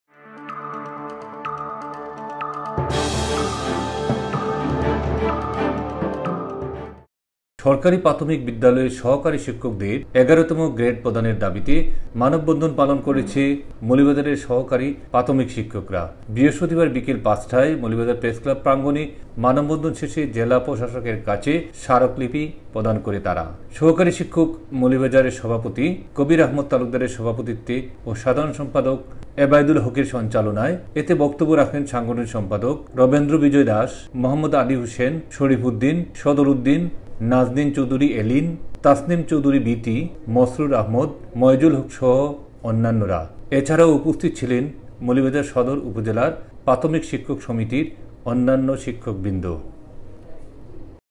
১১তম গ্রেডের দাবিতে মৌলভীবাজার জেলার সহকারী শিক্ষকদের মানববন্ধন
মৌলভীবাজার  শিক্ষকদের মানববন্ধন